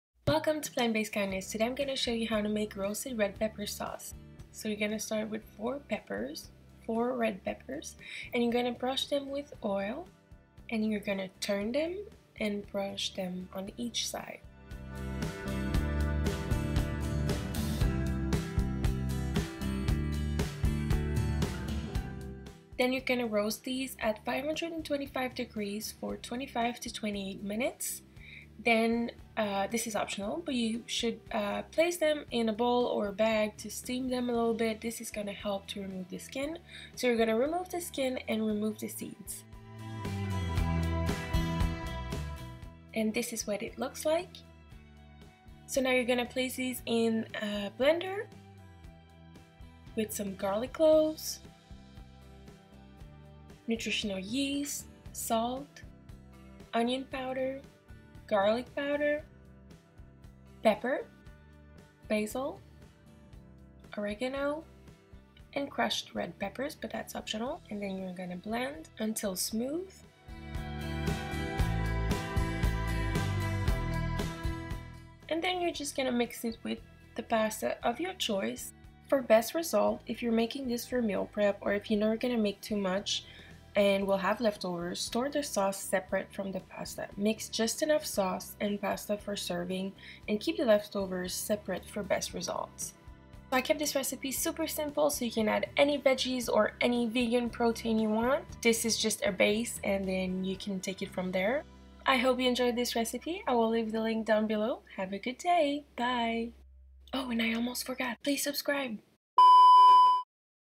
Roasted Red Pepper Sauce
Roasted red pepper pasta sauce recipe. So easy, delicious, nutritious and colorful! I love it!!! It's vegan, gluten-free if you use GF pasta, and you can add so much to make this different each time! Youse the veggies that are begging to be used in your fridge! You can add tempeh, tofu, TVP, beans, lentils or even use chickpea or black bean pasta for protein!

☆Find Everyday Vegan Food on social media☆